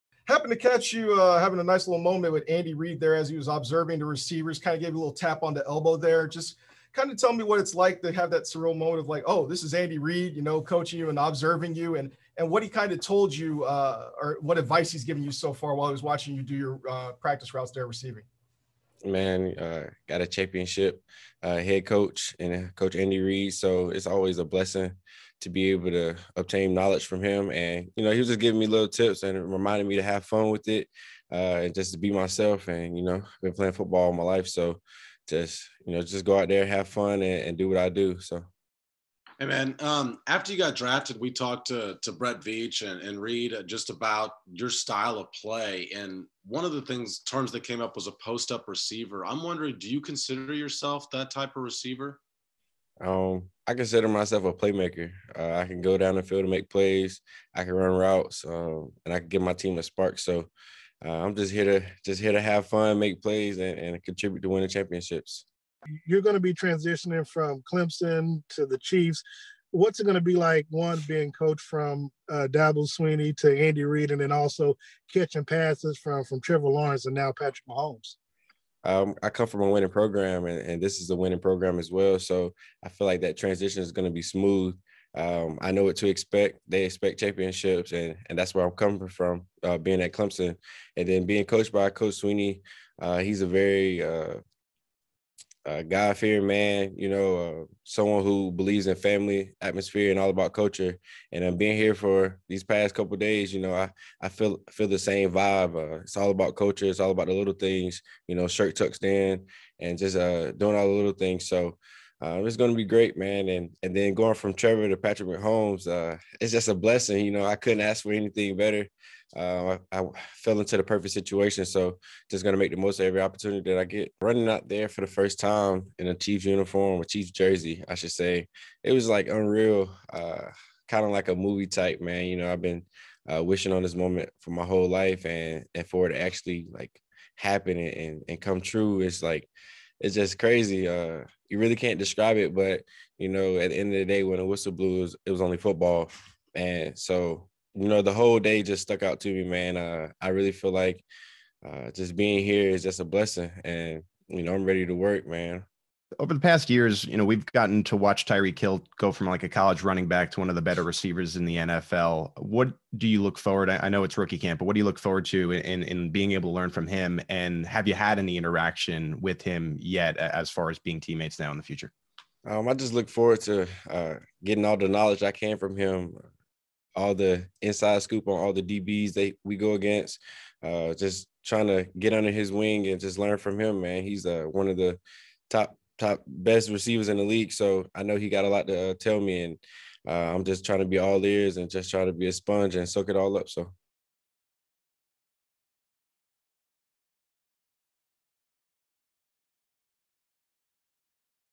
Kansas City Chiefs draft pick Cornell Powell takes part in rookie mini camp
The Kansas City Chiefs drafted Clemson's Cornell Powell in the Round 5 of the 2021 NFL draft. The wide receiver participated in the Chiefs three-day rookie mini camp and talked about learning from Tyreek Hill and working with quarterback Patrick Mahomes during a press conference Saturday, May 15, 2021 in Kansas City, Missouri. More Chiefs coverage

Video by Tammy Ljungblad | The Kansas City Star